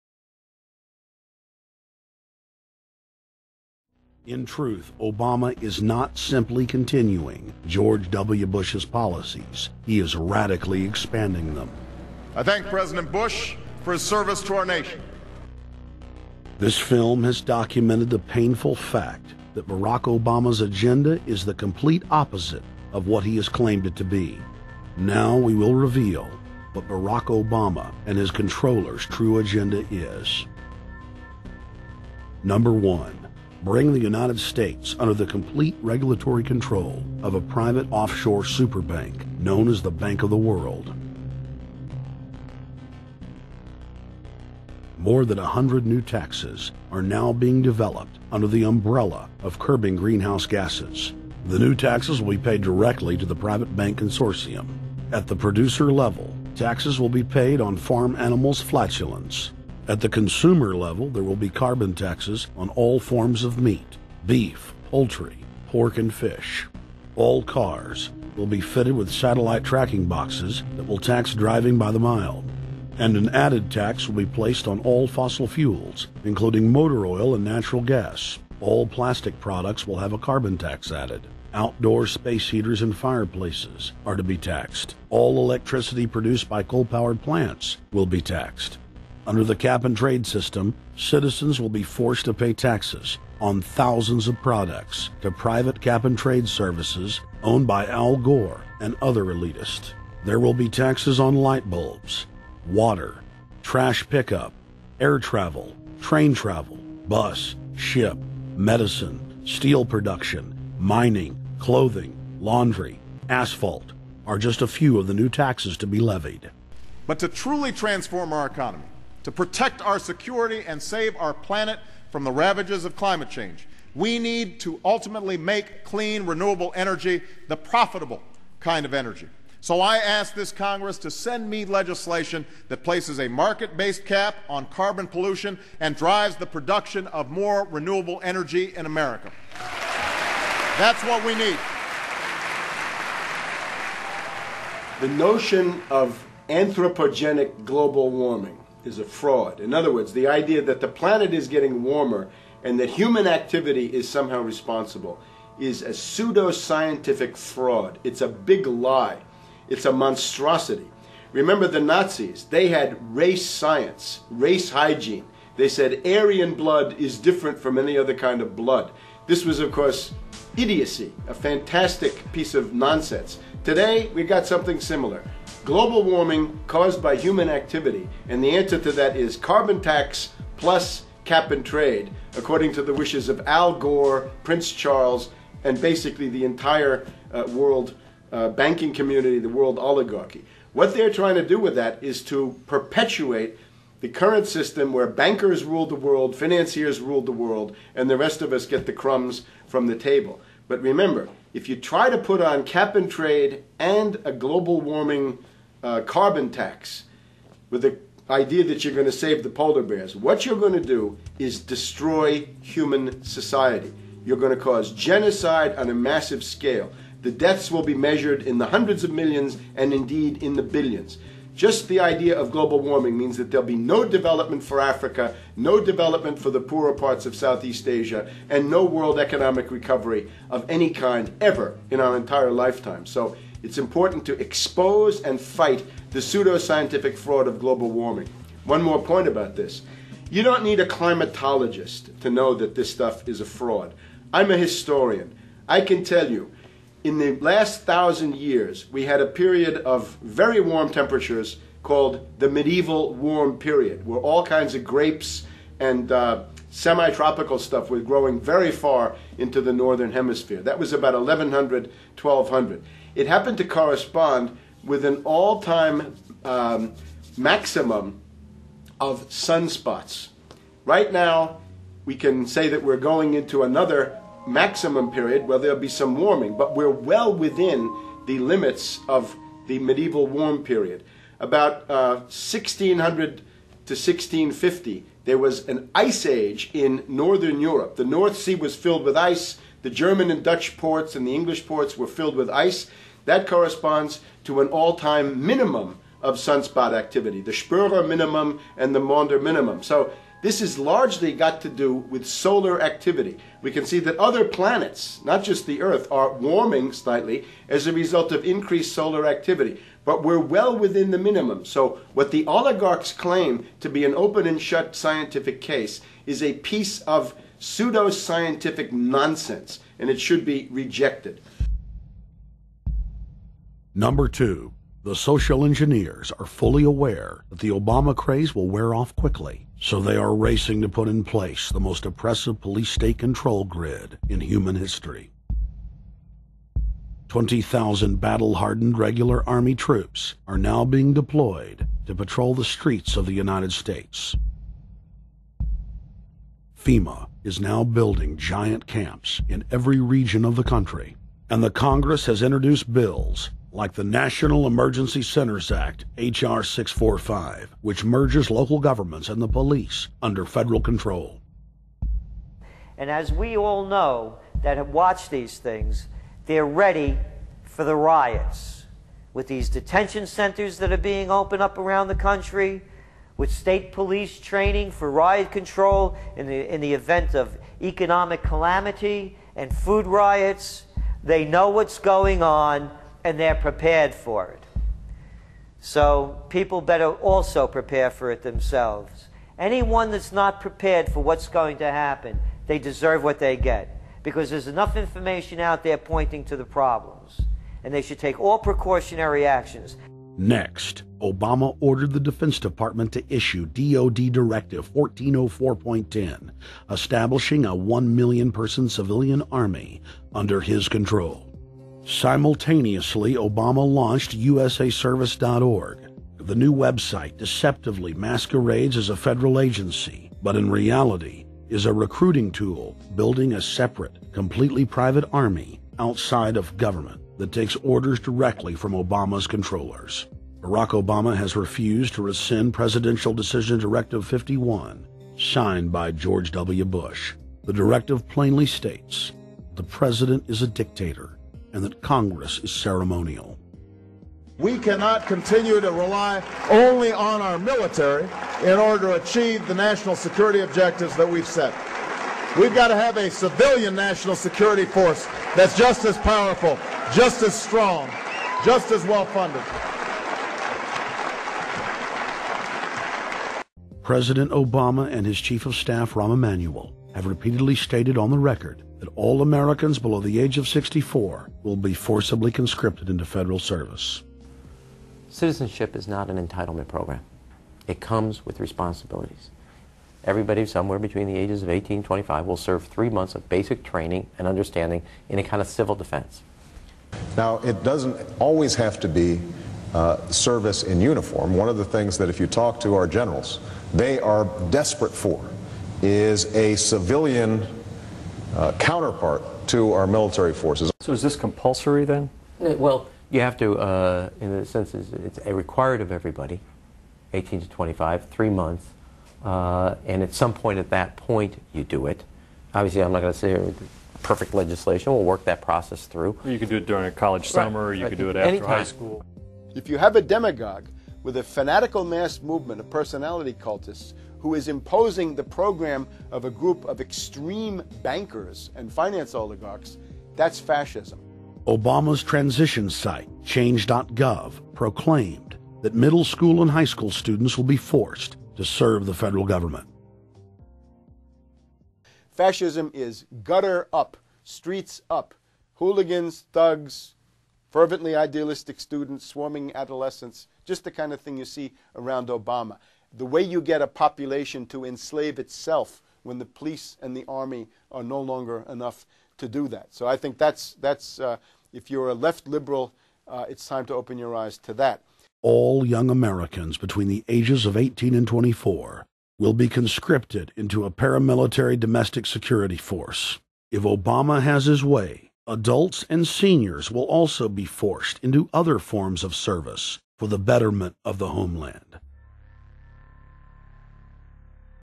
Obama's True Agenda - Part 1
Alex Jones lays out Obama's true agenda...or rather, the agenda of those who back Obama.